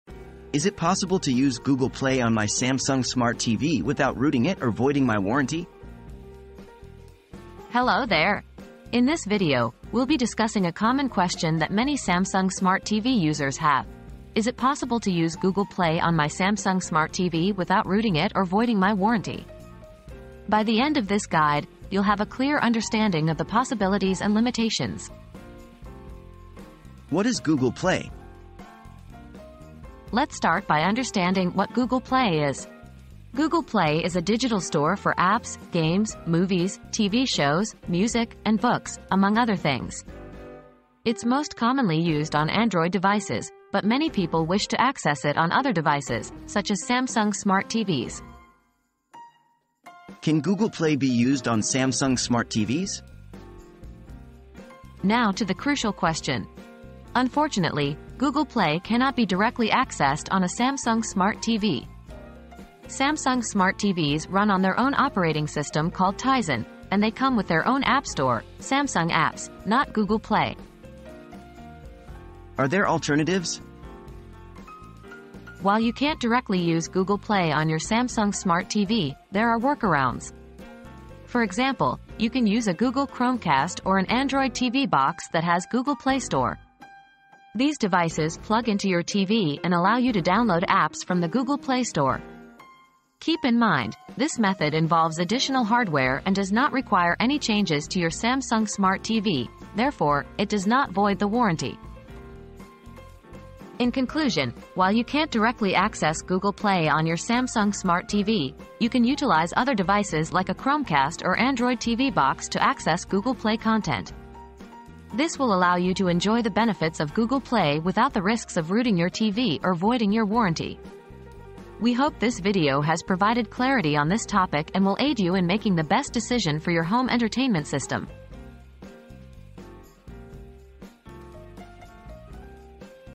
Is it possible to use Google Play on my Samsung Smart TV without rooting it or voiding my warranty?
Using Google Play on Samsung Smart TV: A Simple Guide • Unlock the Power of Google Play on Your Samsung Smart TV - Without Voiding Your Warranty! Watch this Simple Guide Now.

00:00 • Is it possible to use Google Play on my Samsung Smart TV without rooting it or voiding my warranty?
00:27 • What is Google Play?
00:50 • Can Google Play be Used on Samsung Smart TVs?
01:12 • Are There Alternatives?